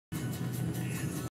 Троллейбусы Москва 26.12.10
Москва 26.12.10, обледенели провода троллейбусов